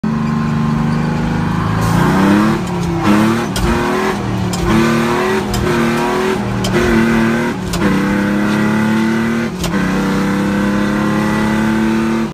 Woman trucker shifting gears extremely fast.
Skilled woman trucker speed shifting gears in a if rig. Have you ever seen a more skilled truck driver?